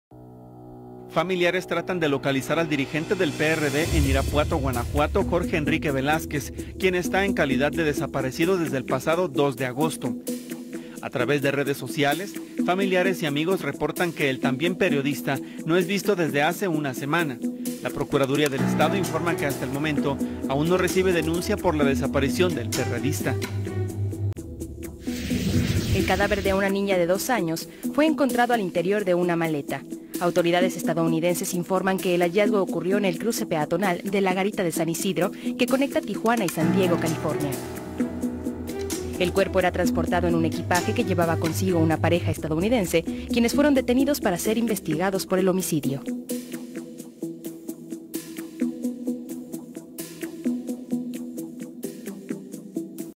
Denuncian desaparición de líder del PRD en Irapuato
El dirigente municipal del Partido de la Revolución Democrática en Irapuato , y quien también se desempeña como periodista, Jorge Enrique Velázquez Delgado, fue reportado como desaparecido por sus familiares.